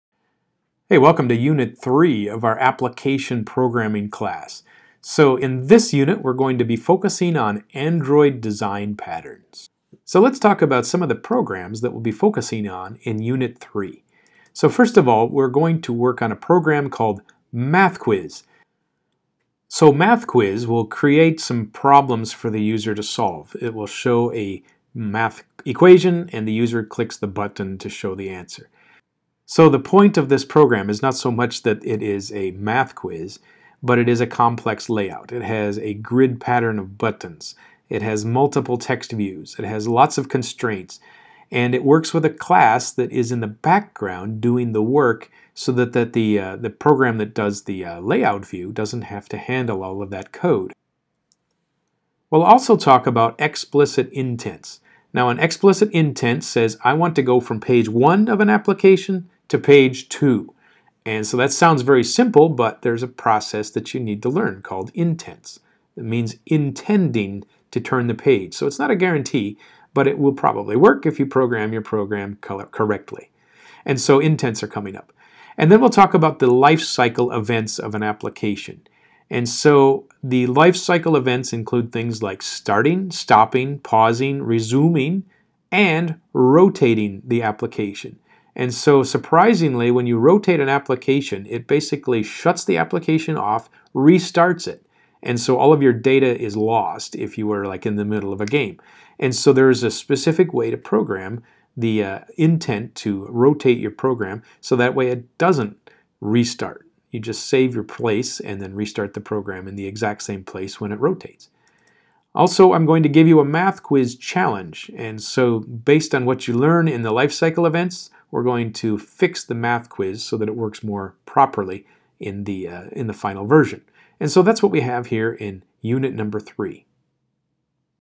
Android App Development Unit 3
Android app development is a semester long course.  Unit 3 focuses on designing apps with MVC, using intents and the lifecycle events of an app.